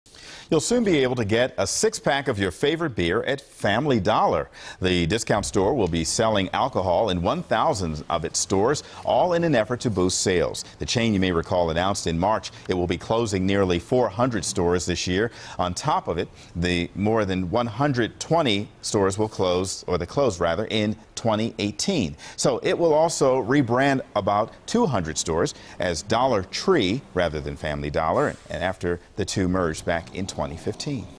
Family Dollar Stores To Begin Selling Alcohol
The discount store said it will be selling alcohol in 1000 of it's stores in an effort to boost sales.